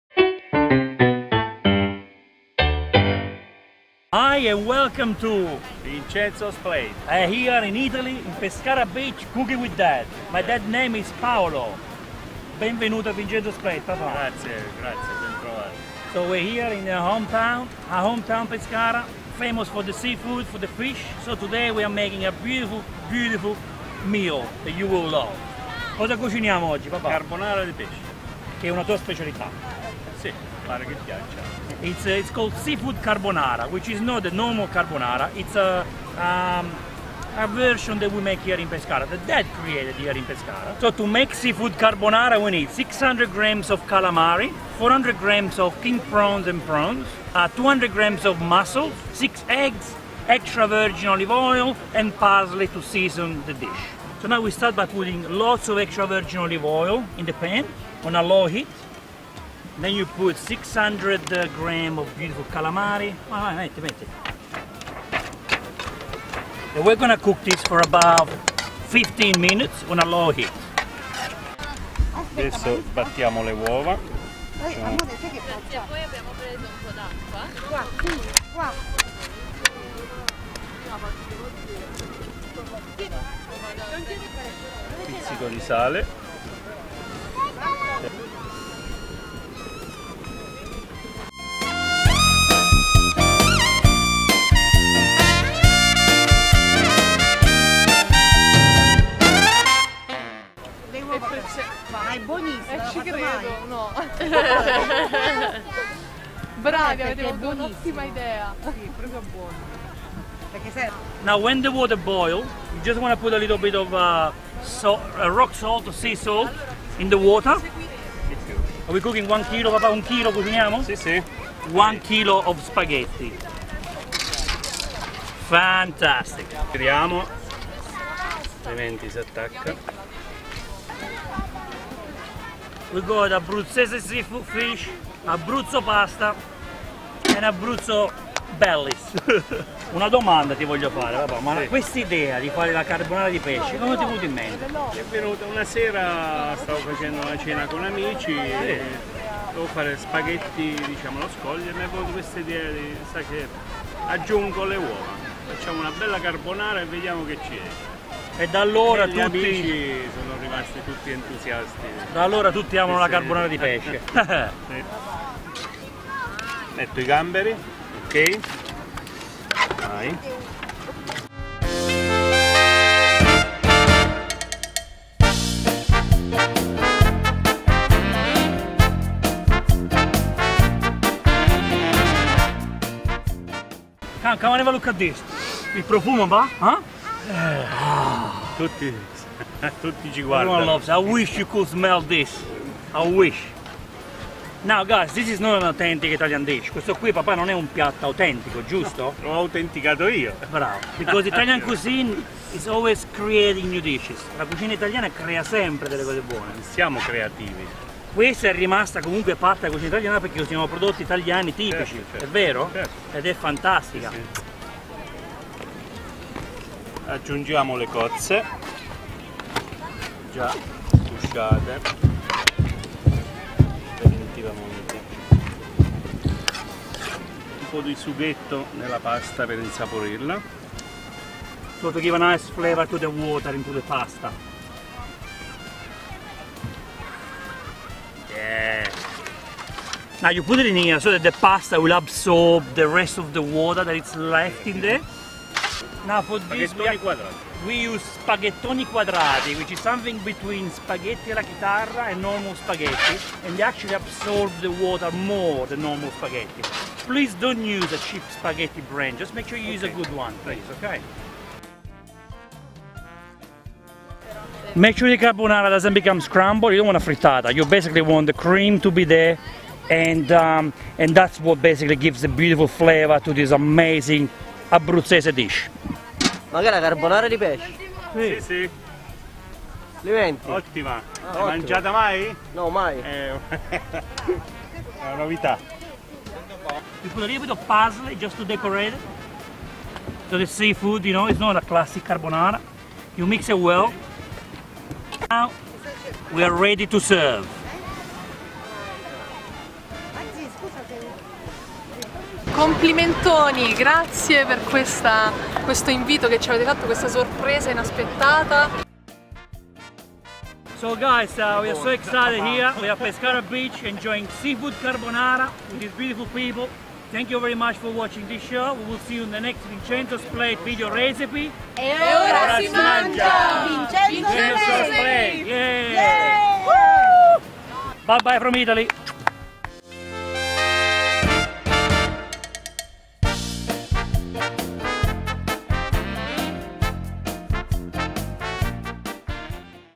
SEAFOOD SPAGHETTI CARBONARA | Seafood Pasta Recipe feat MY DAD
Have you ever heard of Seafood Spaghetti Carbonara ? No? Well now you have!! Watch my first ever video recipe filmed in my home town ‪Pescara‬ , Italy‬ on the ‪BEACH‬ with one of my biggest inspirations...my ‪#‎DAD‬
He is the best self-taught ‪‎seafood‬ cook ever and I can't wait tor you to enjoy the fun we get up to while you learn this original ‪‎DELICIOUS‬ dish.